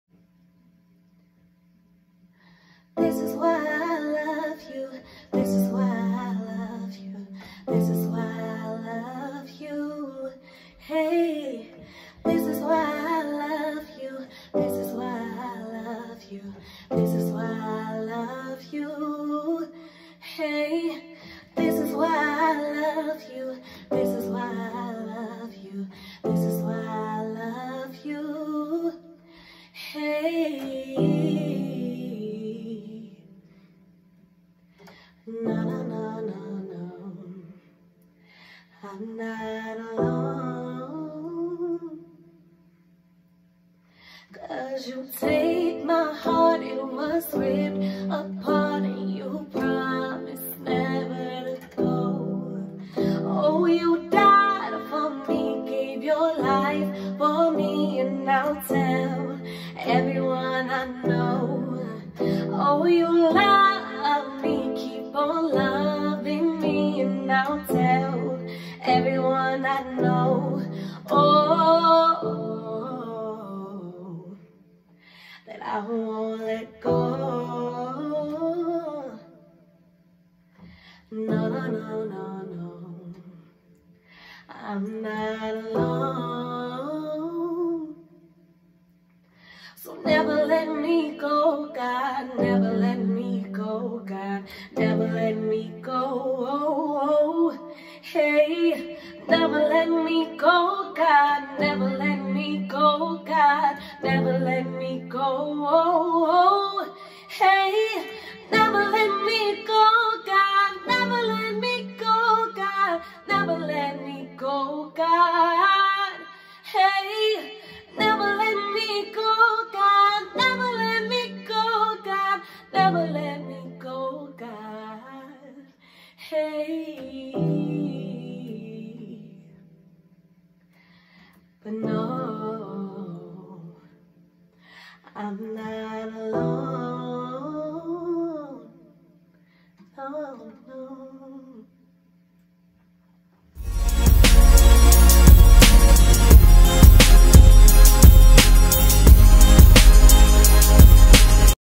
Sharing a song I wrote in under 5 mins 😱
Alexis shares a song that she wrote in less than 5 mins ! Enjoy !